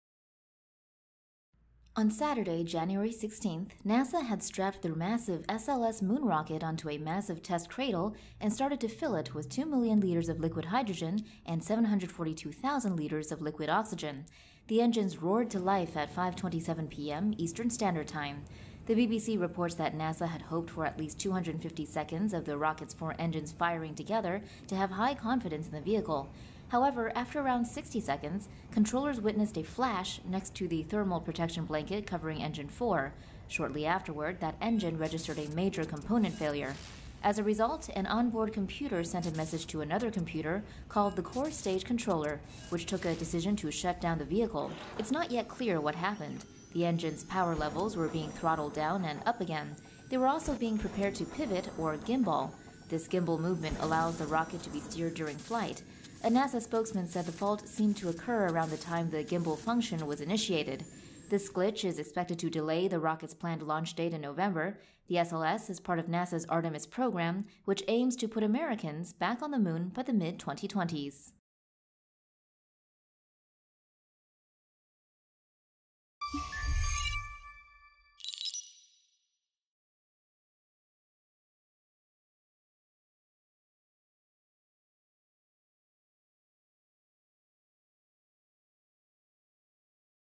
Rocket glitch could be big setback for NASA's moon mission
On Saturday 16 January, NASA had strapped their massive SLS moon rocket onto a massive test cradle and started to fill it with 2 million liters of liquid hydrogen and 742,000 liters of liquid oxygen.

The engines roared to life at 5:27 PM, Eastern Standard Time.

RUNDOWN SHOWS:
1. Show rocket in massive test cradle, liquid hydrogen and oxygen tanks being fueled
2. Engines fire up, timer appears, engineers looking at timers, aiming for 250 seconds
3. Flash seen by engine 4, onboard computer starts flashing red
4. Computer sends message to control computer, which sends shut-down to engine 4
5. Back in time to when all 4 engines firing, thrust variation, gimbal function activates
6. Engines firing while pivoting together, then flash and shutdown sequence

The BBC reports that NASA had hoped for at least 250 seconds of the rocket's four engines firing together to have high confidence in the vehicle.

However, after around 60 seconds, controllers witnessed a flash next to the thermal-protection blanket covering engine four. Shortly afterward, that engine registered a major component failure.

As a result, an onboard computer sent a message to another computer called the core-stage controller, which took a decision to shut down the vehicle.

It's not yet clear what happened. The engines' power levels were being throttled down and up again. They were also being prepared to pivot, or "gimbal". This gimbal movement allows the rocket to be steered during flight.

A NASA spokesman said the fault seemed to occur around the time the gimbal function was initiated.

This glitch is expected to delay the rocket's planned launch date in November.

The SLS is part of NASA's Artemis program, which aims to put Americans back on the moon by the mid 2020s.